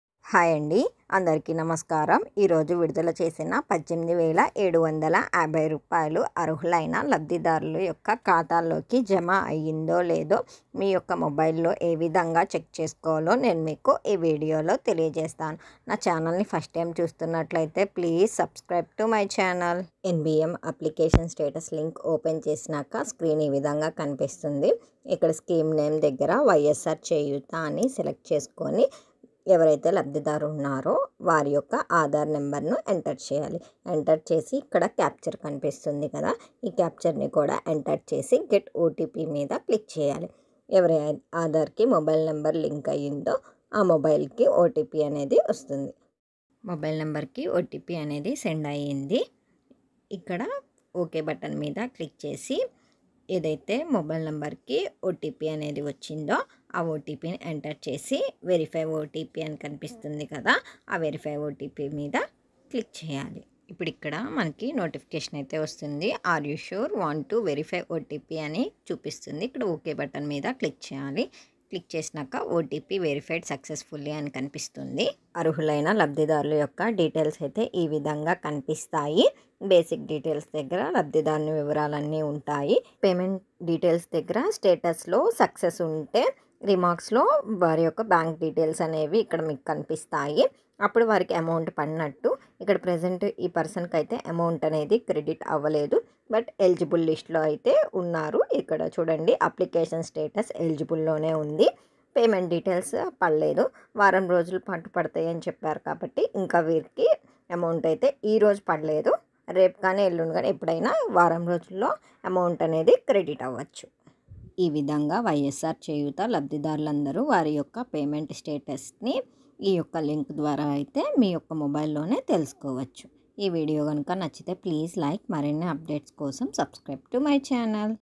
చేయూత డబ్బులు పడుతున్నాయి మీమొబైల్ లో చెక్ చేసుకోండి ysrcheyutha onlinestatusofpayment
వైయస్సార్ చేయూత పథకం ఈరోజు కుప్పంలో ప్రారంభం చేశారు కదా దానికి సంబంధించి అర్హులైన లబ్ధిదారులకు అమౌంట్ వారి ఖాతాలోకి జమ అయ్యిందో లేదో మీయొక్క మొబైల్ లో ఏ విధంగా చెక్ చేసుకోవాలో నేను మీకు ఈ వీడియోలో తెలియజేస్తున్నాను @AgnesTelugu

Following video audio content Making Education Purposes only. Not offense anyone